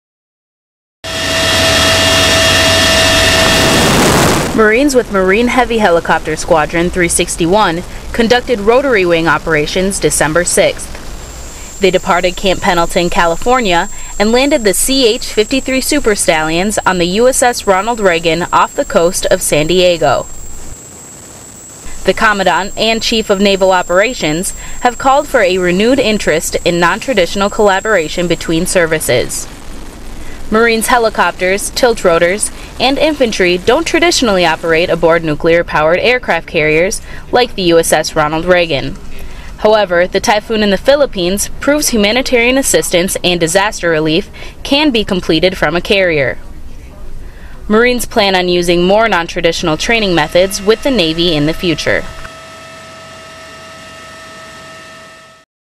Marines Conduct Rotary-Wing Operations aboard the USS Ronald Reagan
Marines with Marine Heavy Helicopter Squadron 361 departed Camp Pendleton, Calif., conduct rotary-wing operations aboard the USS Ronald Reagan Dec. 6. Marines flew CH-53E Super Stallions, the largest and heaviest helicopters in the United States military, from Camp Pendleton, and landed on the aircraft carrier off the coast of San Diego. Video by Lance Cpl. Anna Albrecht | I Marine Expeditionary Force

AiirSource - Thumbs up for the troops!
Your source for current- and archival military/aviation videos.